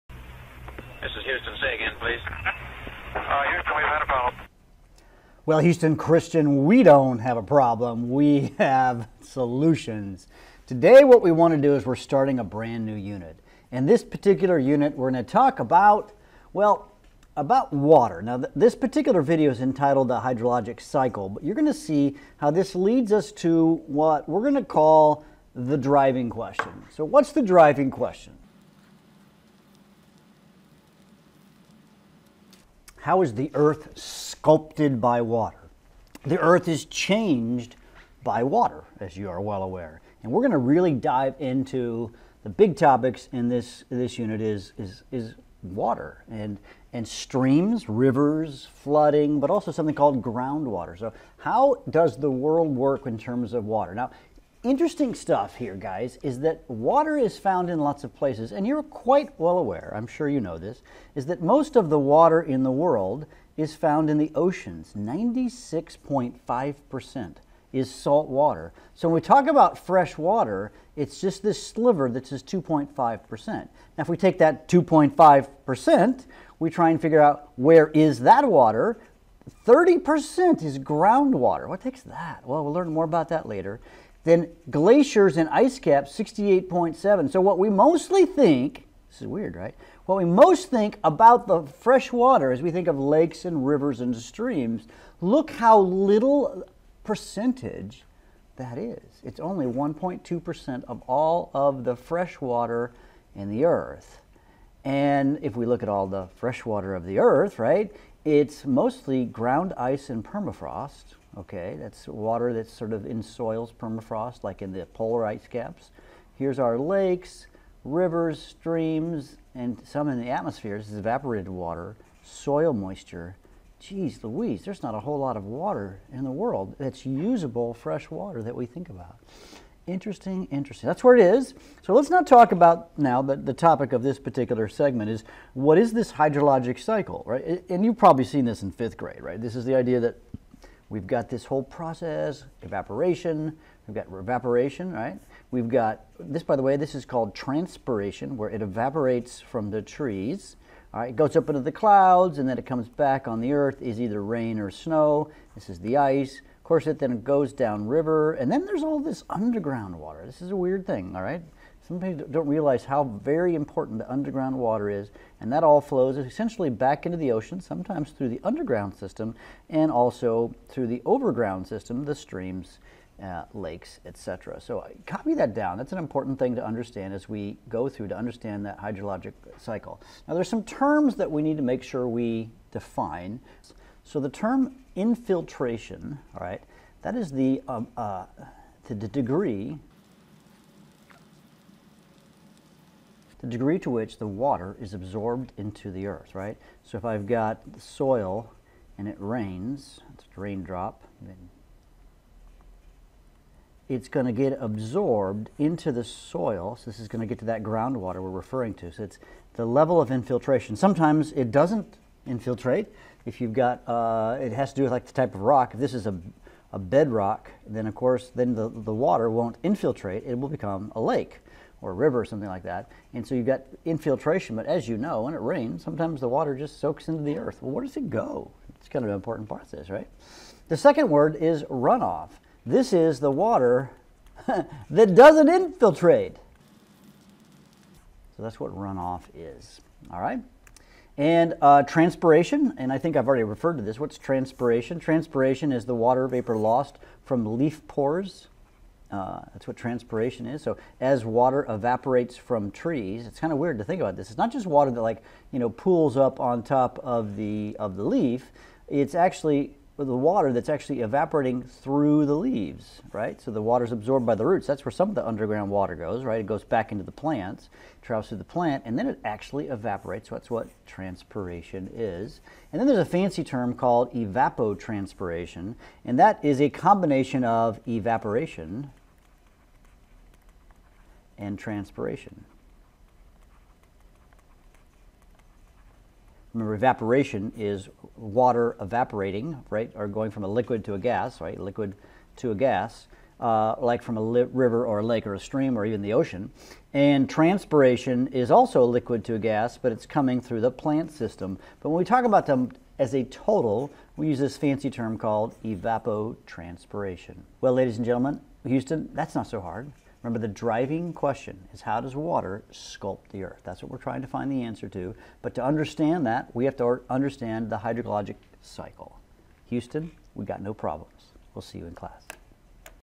The Water Cycle Easily Explained!
The hydrologic cycle is both complex and wondrous. Learn about the wonder of our natural world and be amazed

This is video 1 in an 11 part series.

Award-winning teacher Jon Bergmann Created this series on Running Water for his high school students in their Flipped-Mastery classroom.